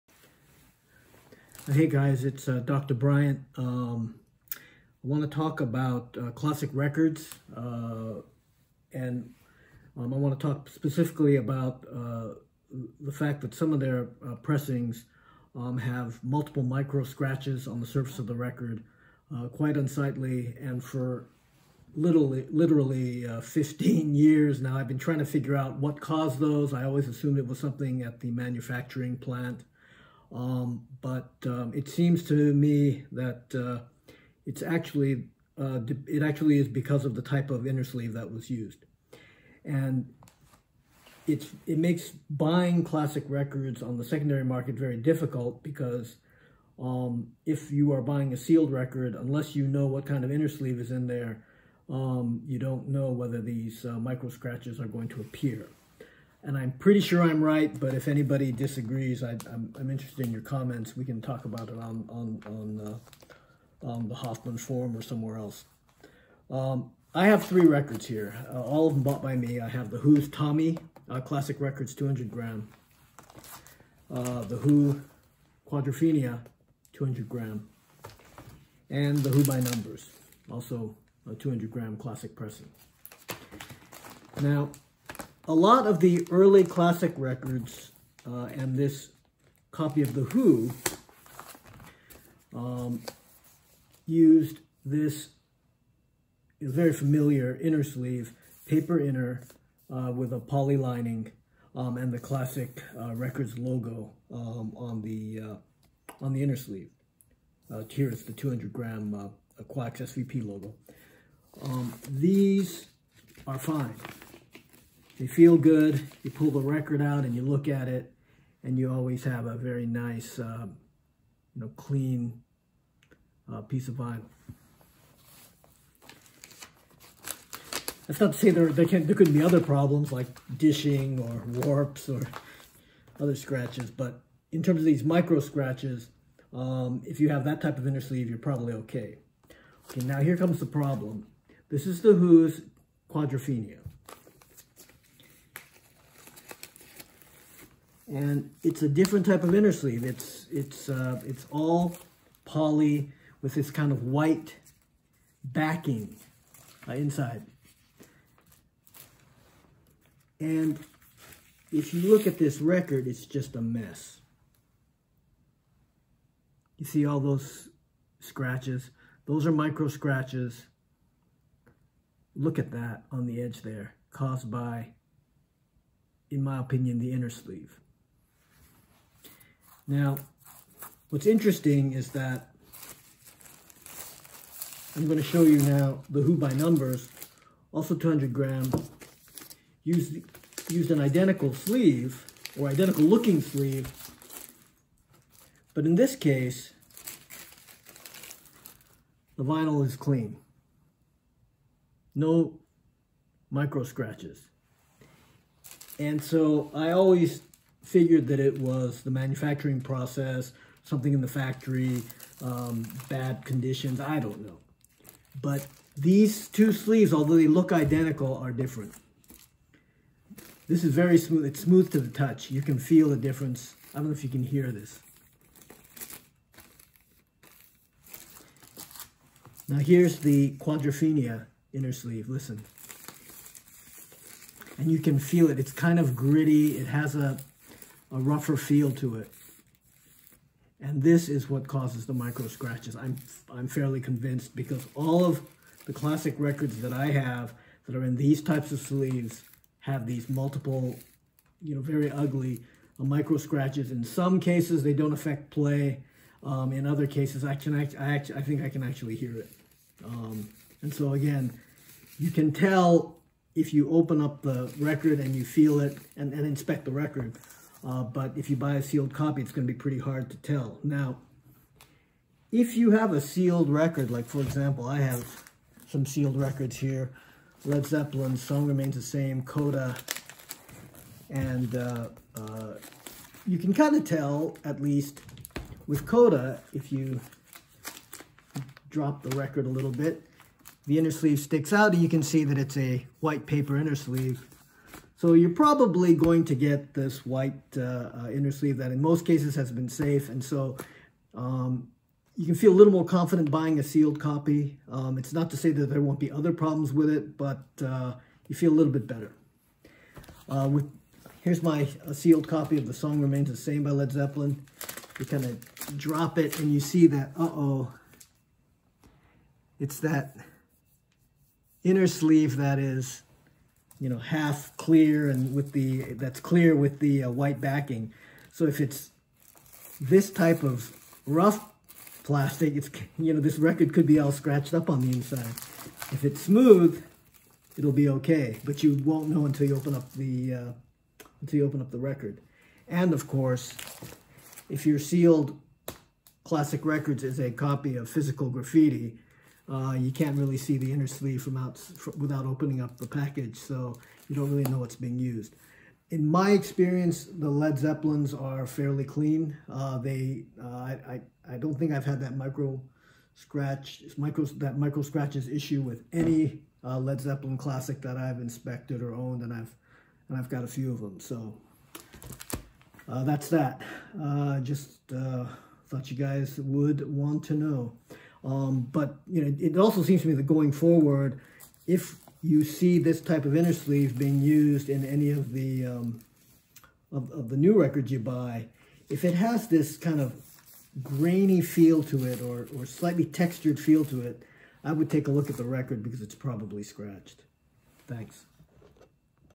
Micro scratches on Classic Records appear to be caused by Inner Sleeves.
If you have these sleeves, replace them.